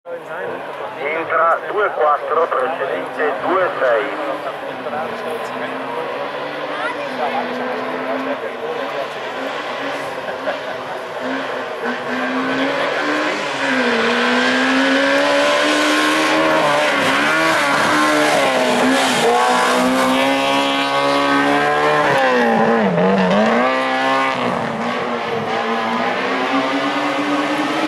1° Bergslalom Vintl-Terenten '19 / Giovannini Angelo / Peugeot 205 Rally / 2° Manche
Organisiert von RacingTeamMeran und MC-Brixen, mit Unterstützung der FF-Vintl und unserem Hauptsponsor Auto Hofer